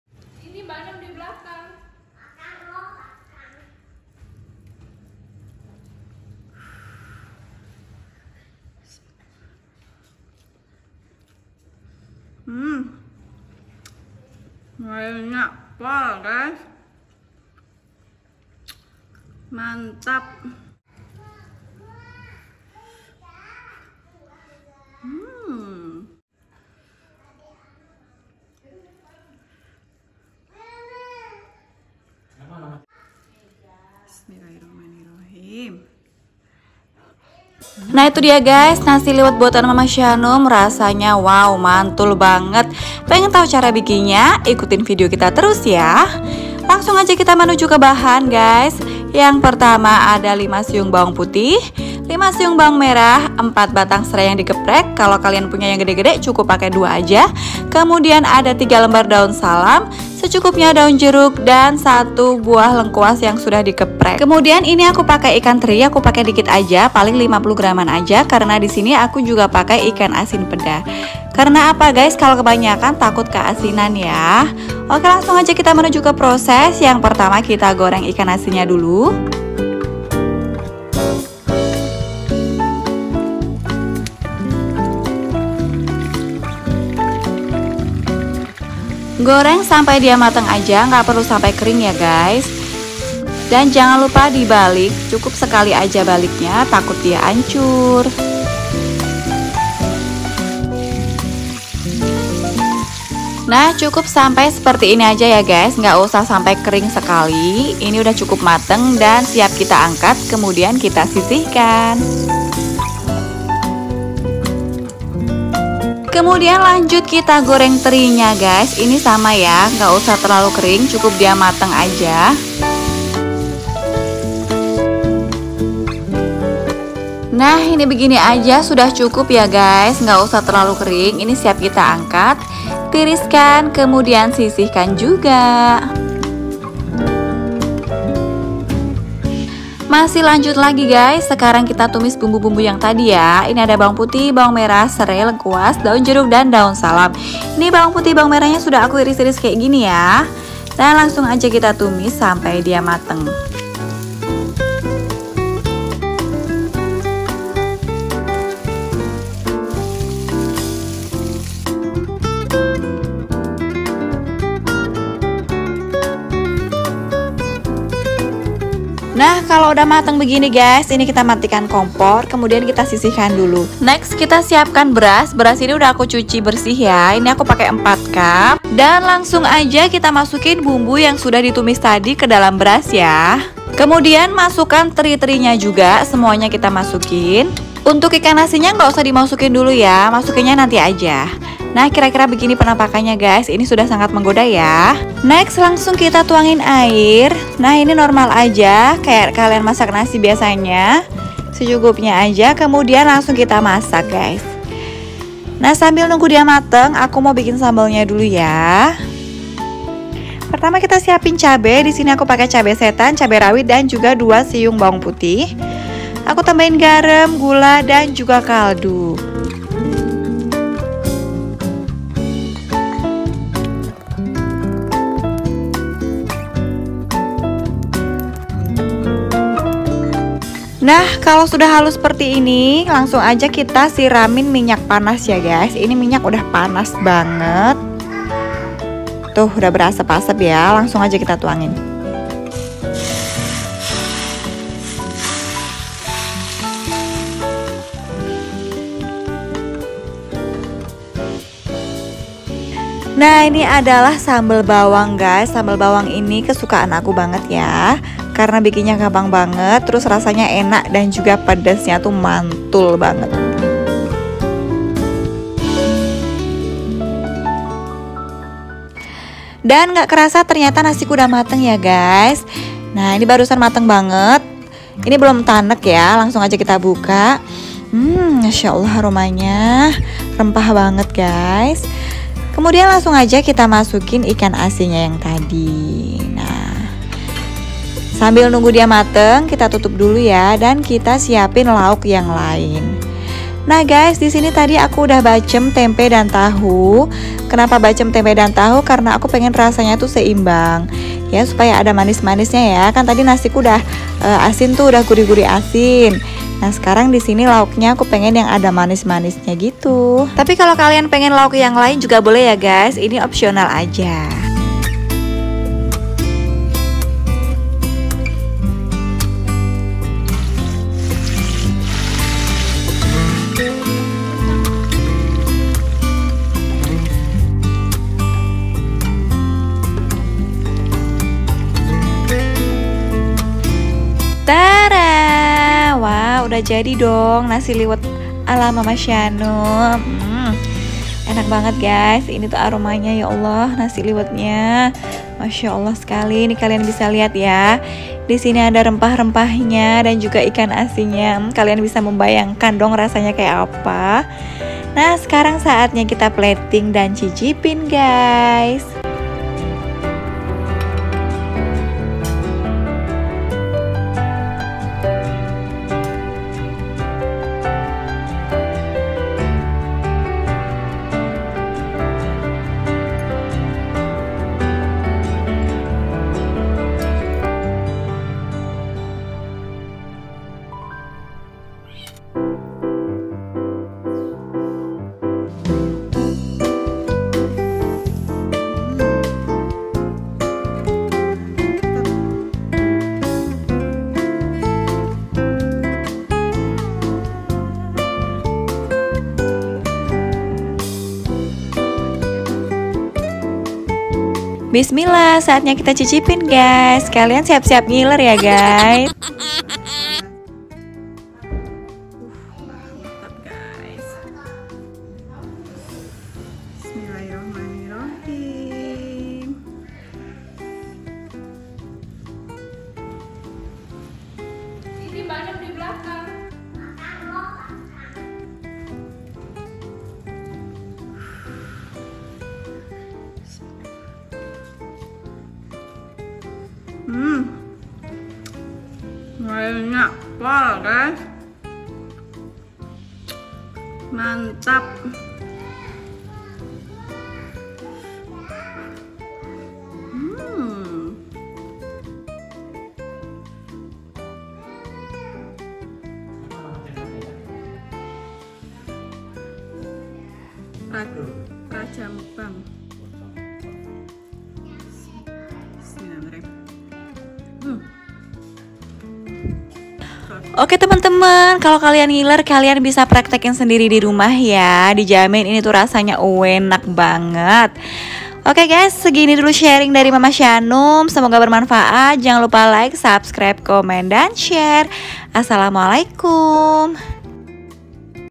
MAMA SHANUM MASAK NASI LIWET RICE COOKER dan SAMBAL BAWANG Enak Mantap‼️
Mama Shanum weekend mask nasi liwet nih bun..
Alhamdulillah keluarga semua suka banget. Bunda kalo mau tau resepnya nih aku bagi ya.

Nasi liwet :
- 4 cup beras
- 5 siung bamer
- 5 siung baput
- 2 batang serai geprek
- 1 buah lengkuas geprak
- 3 lembar daun salam
- 3 lembar daun jeruk
- 50 gram teri
- 2 ekor ikan asin peda

Sambal Bawang
- 7 buah cabe setan
- 10 buah cabe rawit
- 2 siung baput
- garam, gula, kaldu
- 4 sdm minyak panas

Untuk cara-caranya lihat di video ya bunda..
Terimakasih